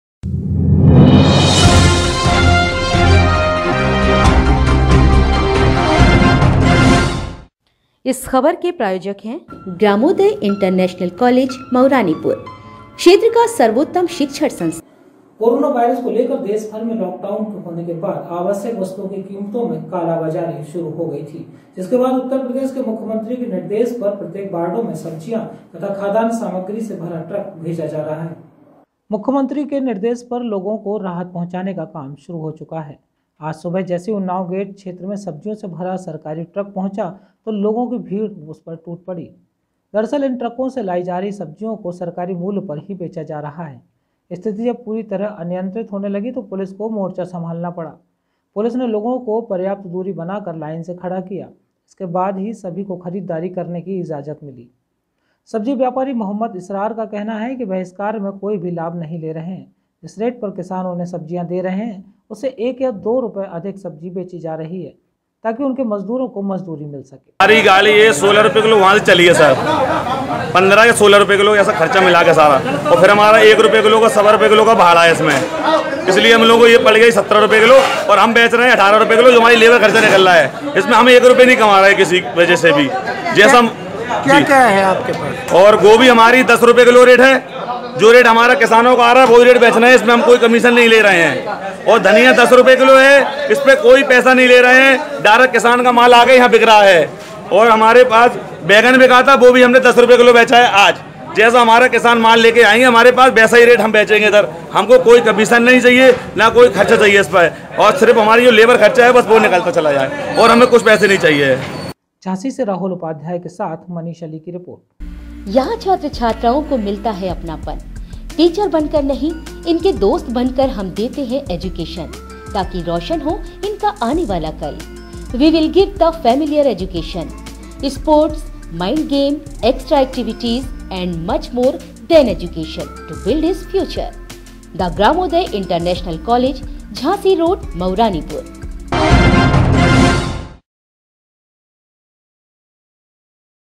मुख्यमंत्री के निर्देश पर हर जगह ट्रकों से सप्लाई होगी सब्जीयां व जरुरी सामान।
After the lock-down across the country due to Corona virus, the prices of essential commodities started getting black marketed. After which a truck full of vegetables and food items is being sent to each ward on the instructions of the Chief Minister of Uttar Pradesh.